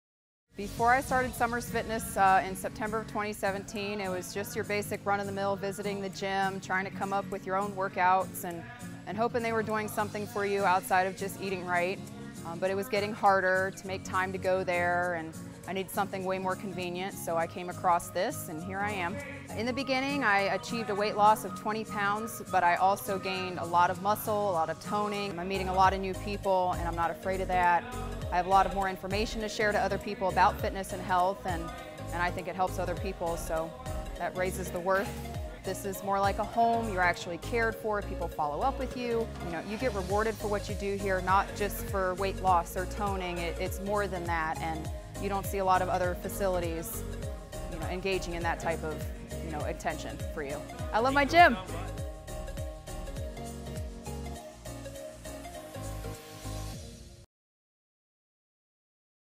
MEET CHERRY
MEET CHERRY: "I was just going to the gym, doing workouts, and hoping they would do something. Now, I've lost 20 pounds and I've met a lot of great people. It's more than a gym, it's a family that cares."